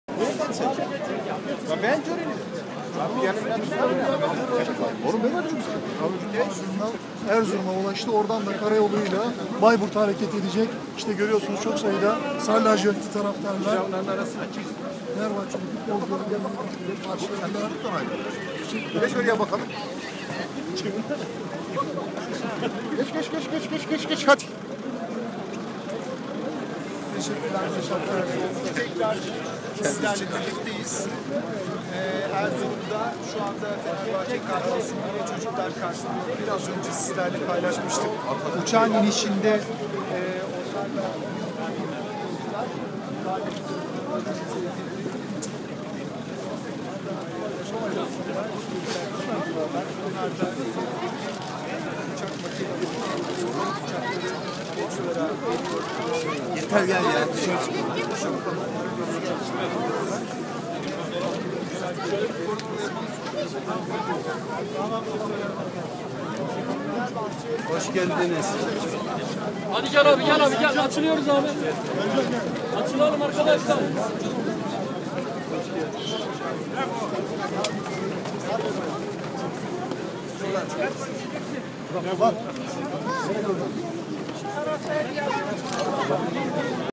Fenerbahçe'nin Bayburt yolculuğu (2)
Bayburt Grup Özel İdare Gençlikspor ile karşılaşacak Fenerbahçe futbol takımı Erzurum'dan Bayburt'a hareket ediyor...